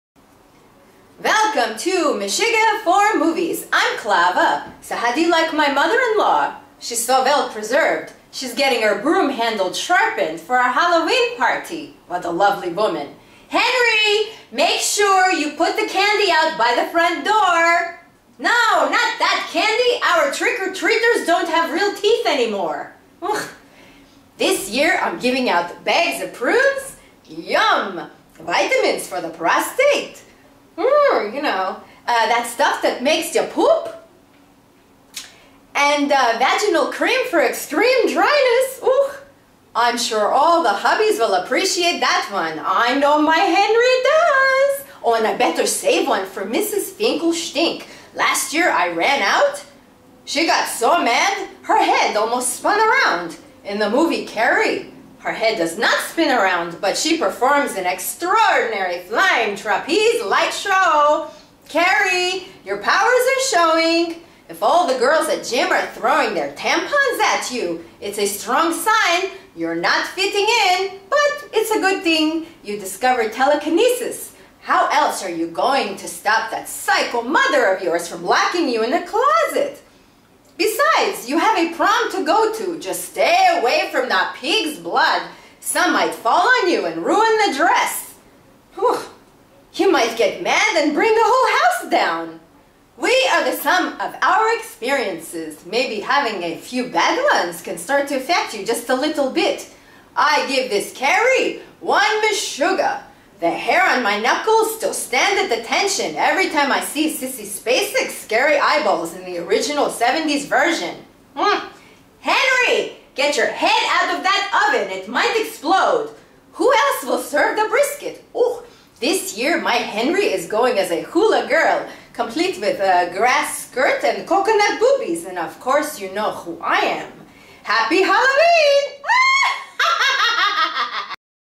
Carrie Meshuga Movie Review
If all the girls at gym are throwing their tampons at you, it's a strong sign you're not fitting in. Carrie, your powers are showing.